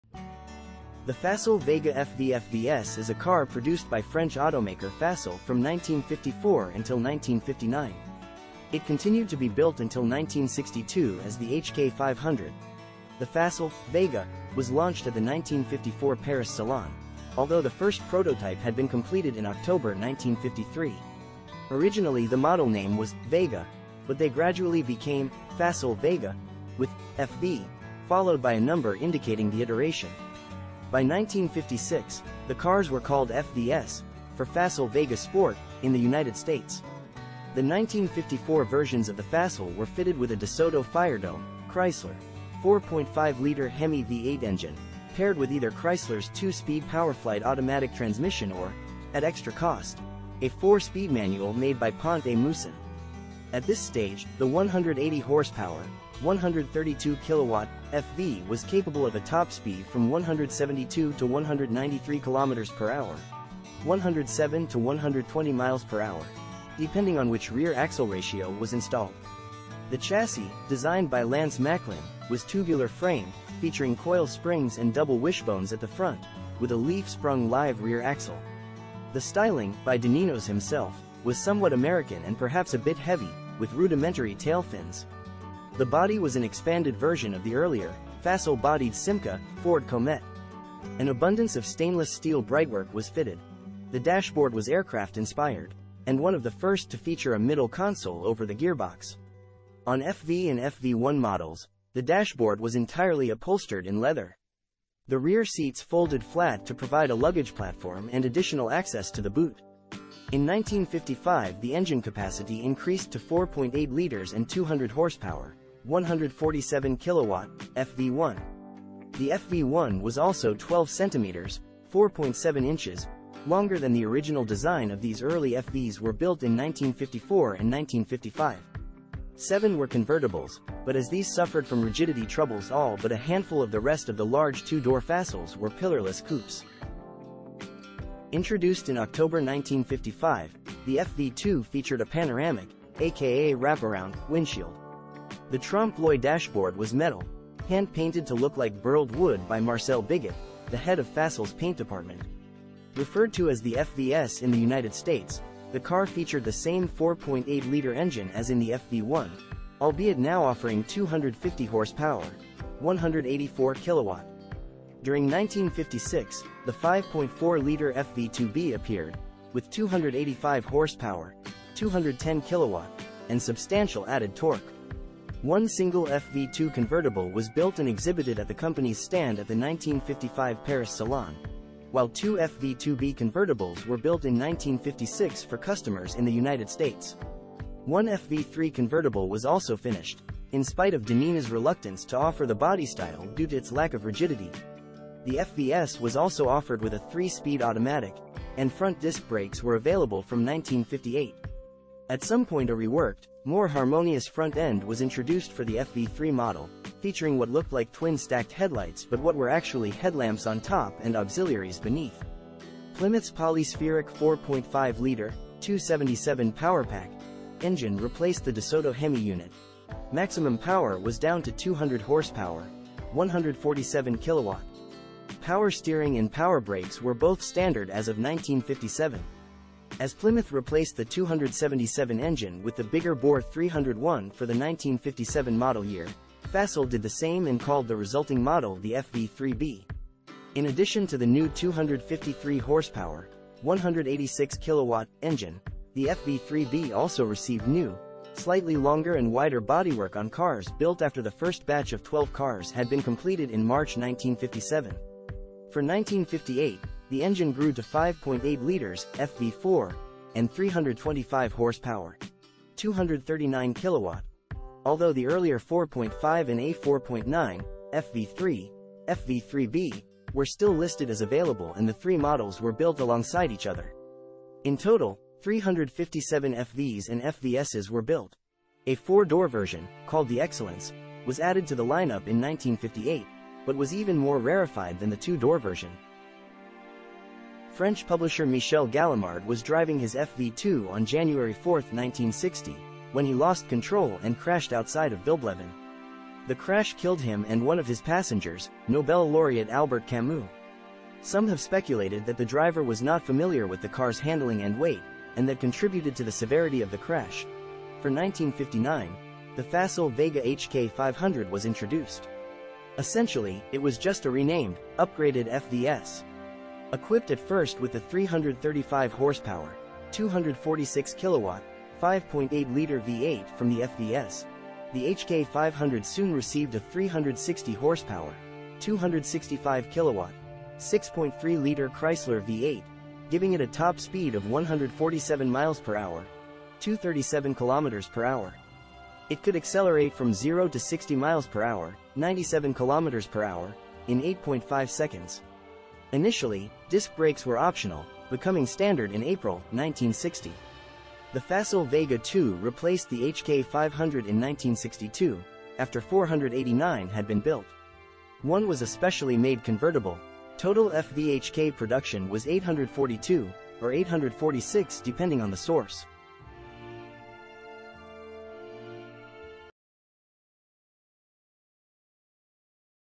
Facel Vega FV
The Facel Vega FV/FVS is a car produced by French automaker Facel from 1954 until 1959. It continued to be built until 1962 as the HK500.